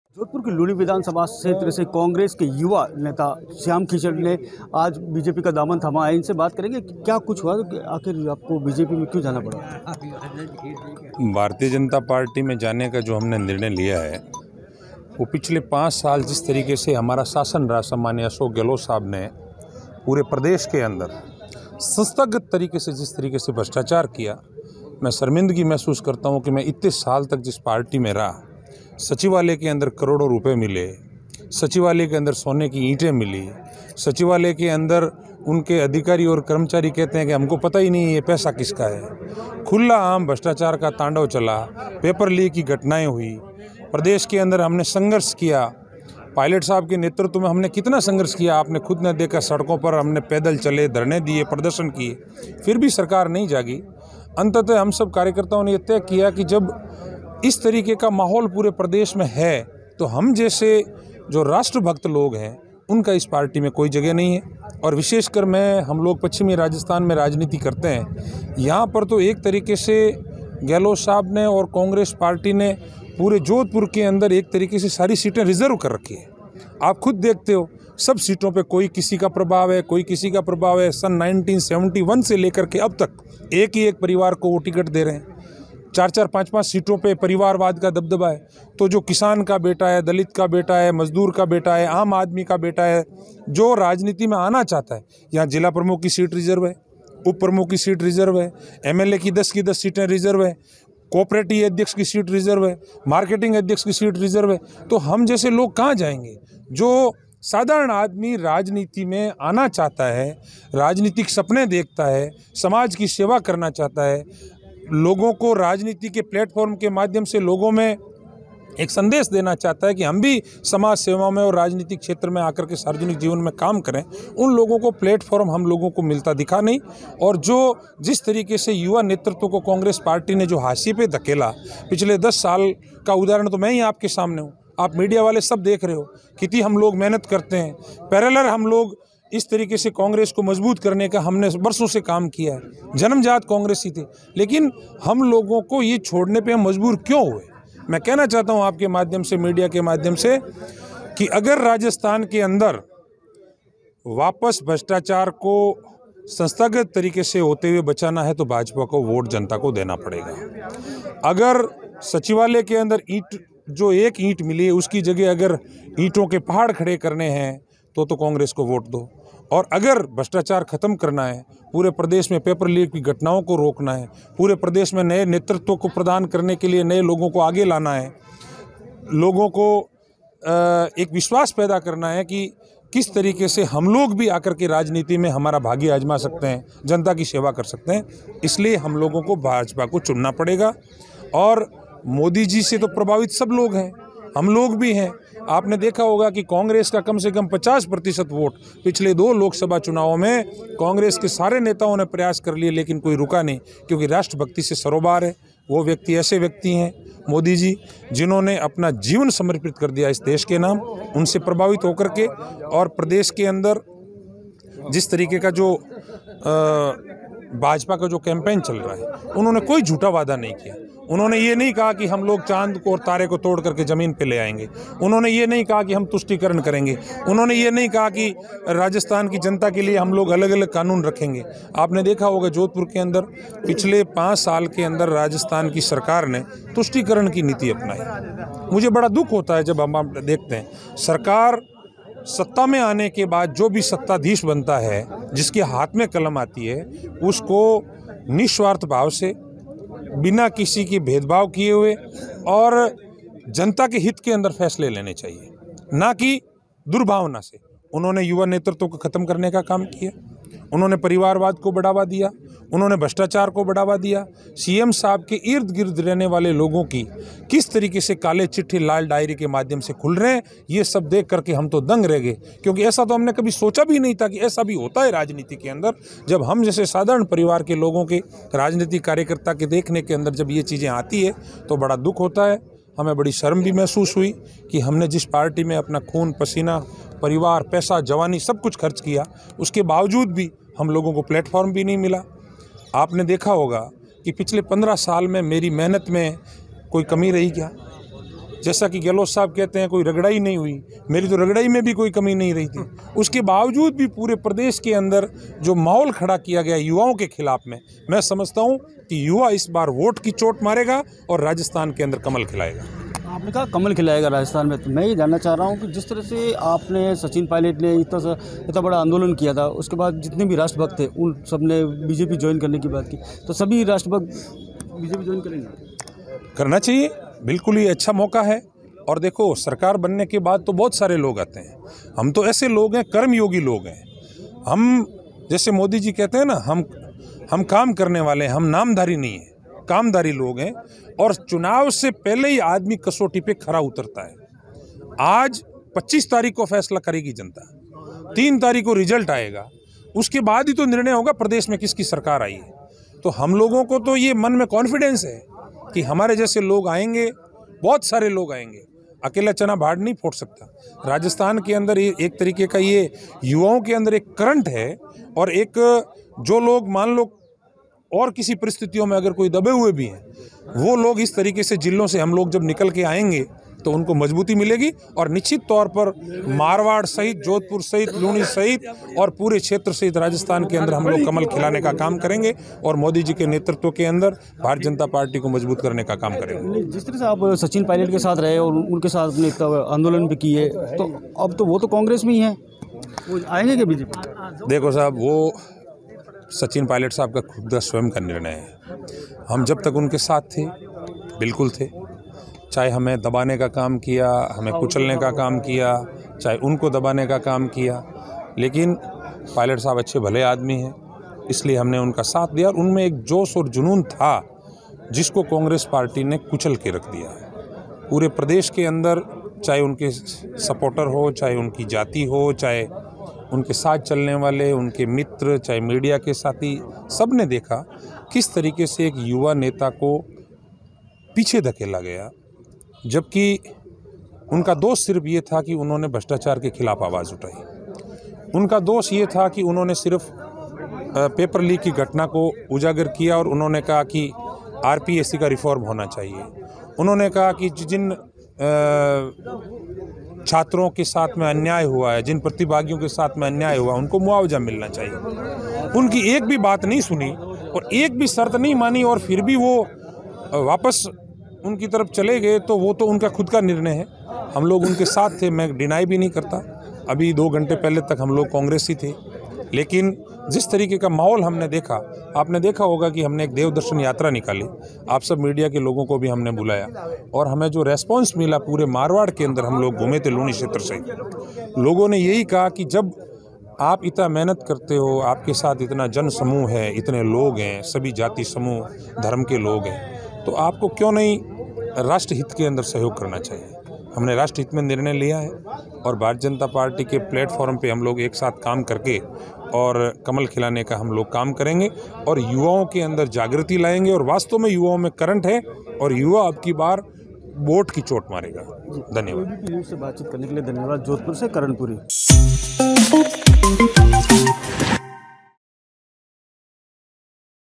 Rajasthan Elections 2023: Sachin Pilot के करीबी ने कांग्रेस को दिया झटका, बीजेपी की JOIN | ABPLIVE
विधानसभा चुनाव में कांग्रेस का टिकट नहीं मिलने के बाद कांग्रेस के लूणी विधानसभा क्षेत्र से युवा नेता श्याम खीचड़ ने कांग्रेस का दामन छोड़ भाजपा का दामन थामा बता दे कि सचिन पायलट के गरीबी रह चुके युवा नेता श्याम की चढ़ने कांग्रेस पर गंभीर आरोप भी लगाए